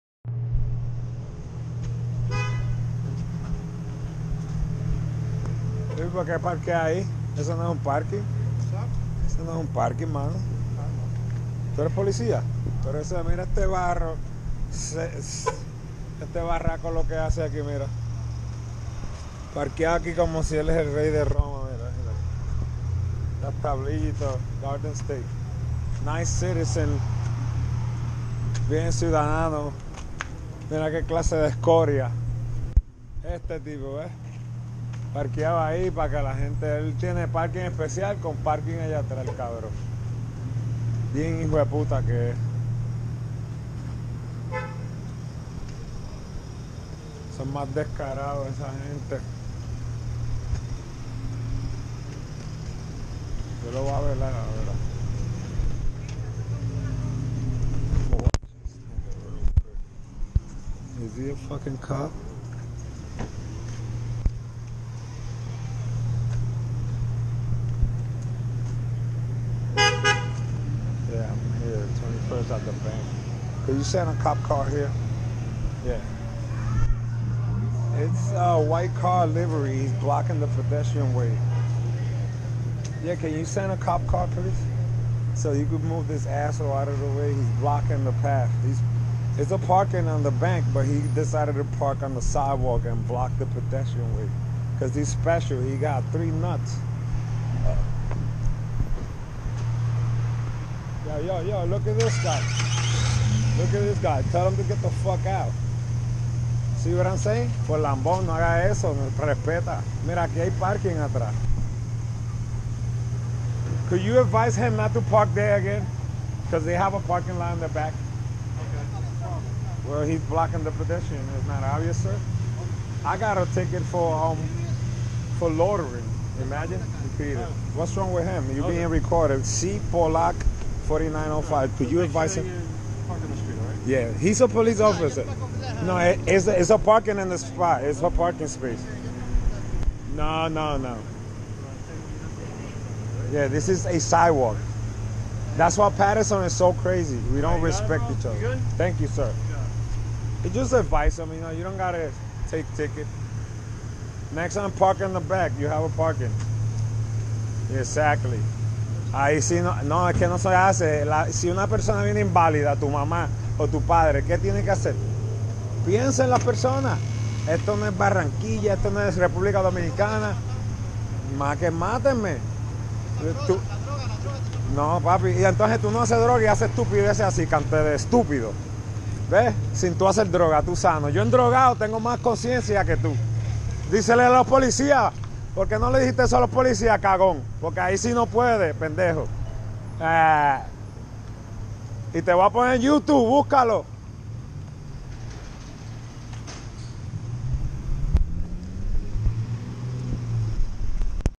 September 3, 2018
Blocking The Pedestrian path Paterson TD Bank big problem everybody wants to double Park here and break the law big inconvenience for the community TD Bank